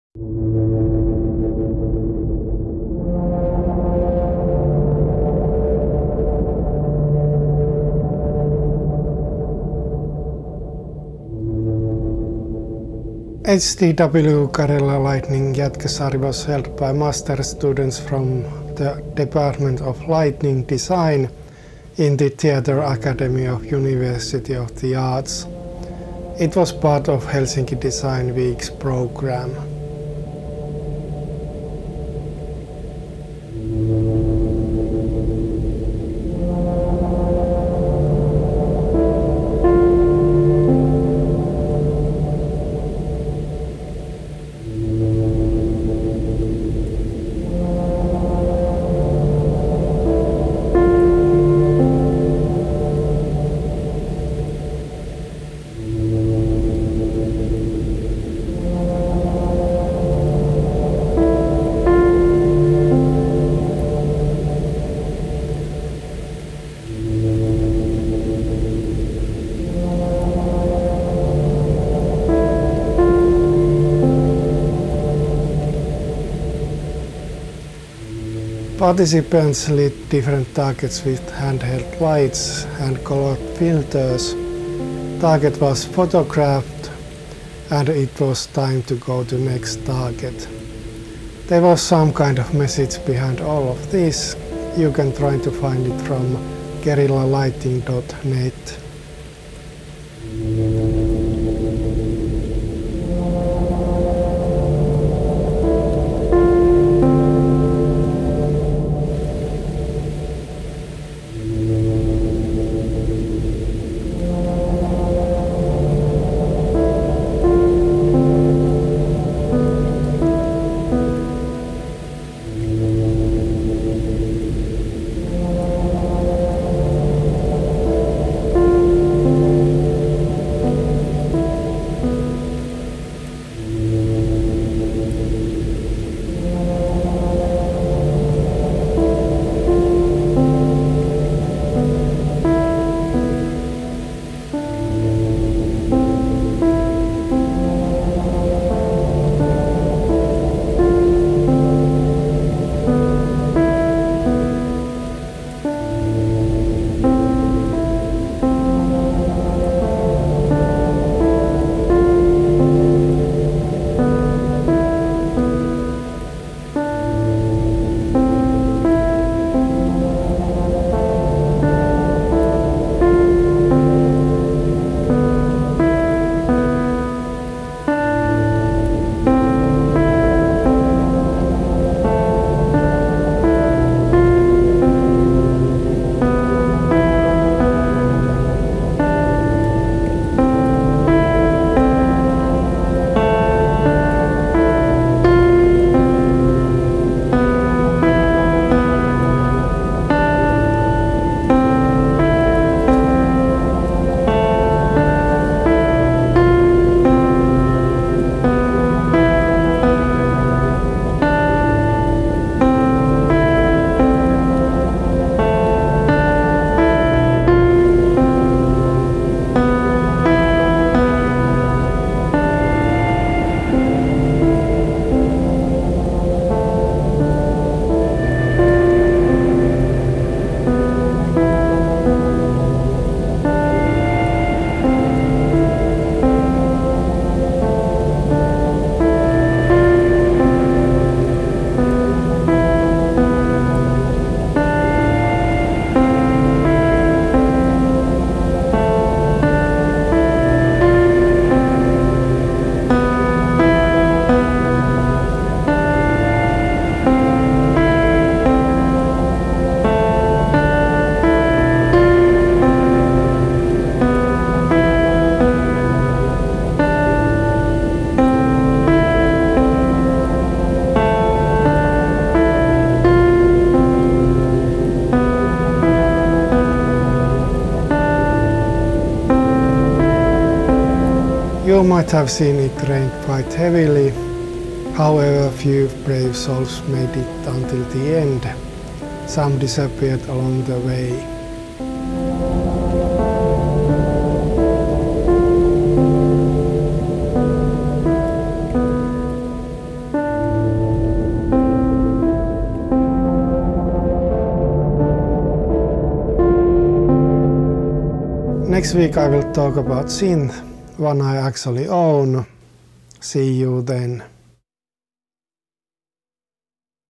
HDW Guerrilla Lighting Jätkäsaari
HDW Guerrilla Lighting Jätkäsaari was held by master students from the department of Lighting Design, in The Theatre Academy of the University of the Arts. It was part of Helsinki Desing Week's program.

Participants lit different targets with handheld lights and colored filters. Target was photographed. And it was time to go to next target. There was some kind of message behind all of this.

You might have seen it rained quite heavily. How ever few brave souls made it until the end. Some disappeared along the way.